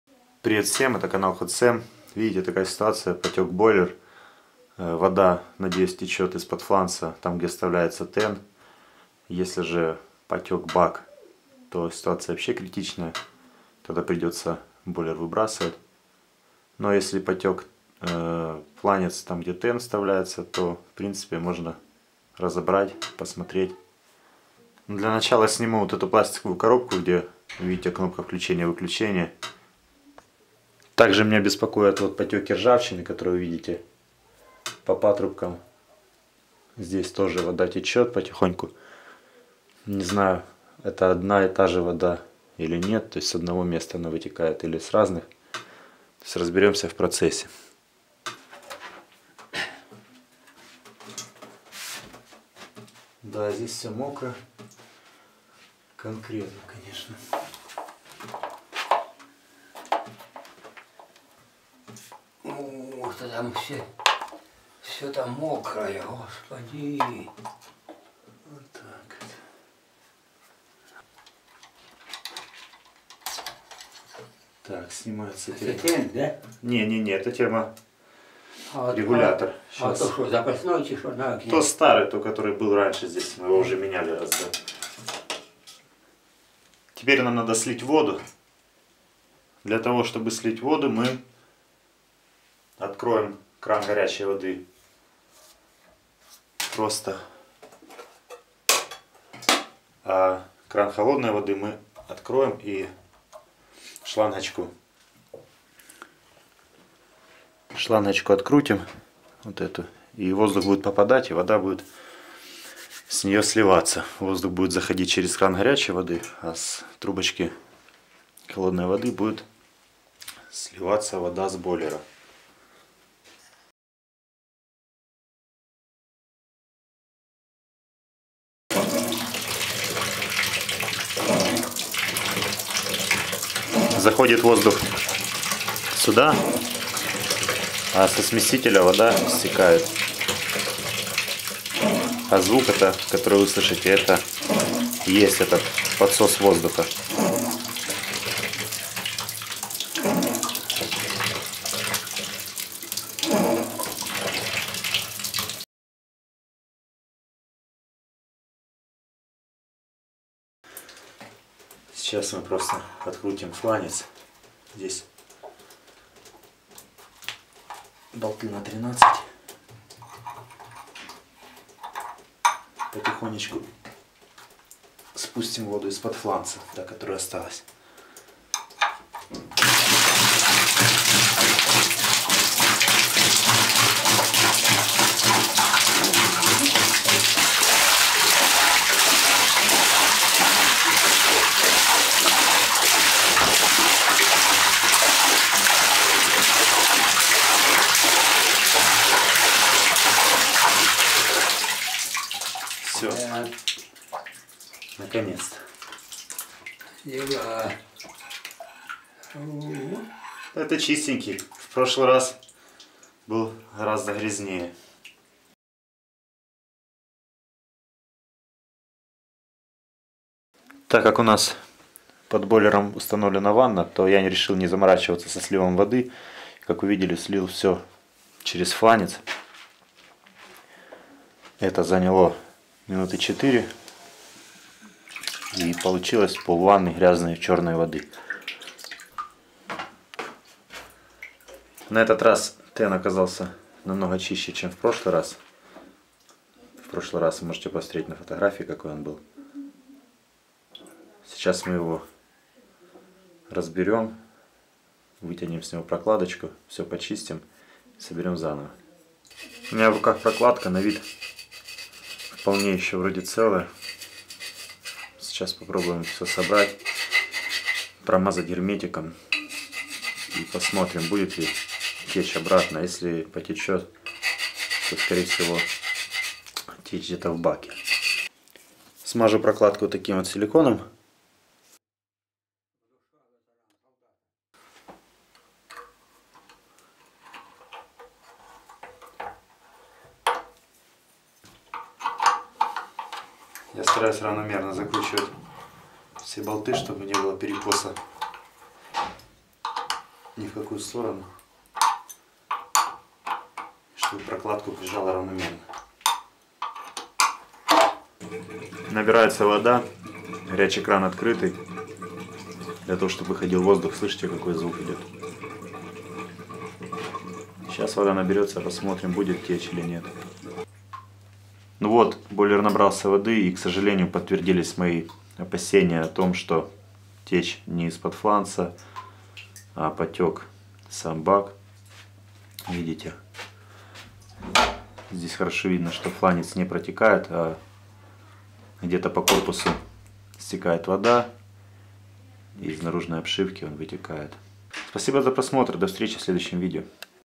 Потек бойлер. Неудачная попытка ремонта водонагревателя
У бабушки в селе потек старый бойлер RAINFORD после семи лет эксплуатации. Сначала я надеялся,что протекла прокладка, там где прикручивается ТЭН - так уже было два года назад. Тогда просто ТЭН был снят и заново посажен на герметик. Течь перестало. В этот раз оказалось, что потек бак, но сначала это диагностировать не удалось, т.к. все было мокрым и складывалось вечатление что течет со всех щелей. После того как тен был снят и установлен заново, стало очевидно, что он не протекает. Потек бак бойлера. На этом срок эксплуатации водонагревателя окончен - необходимо ставить новый бойлер.
ОСНОВНОЙ ОШИБКОЙ,я считаю, было то, что два года назад при снятии ТЭНа мы не заменили магниевый анод. На фото того времени видно, что от магниевого сплава оставался маленький кусочек. А сейчас остался только стальной сердечник. Думаю, если бы тогда был установлен новый магниевый анод - срок службы бойлера был бы увеличен.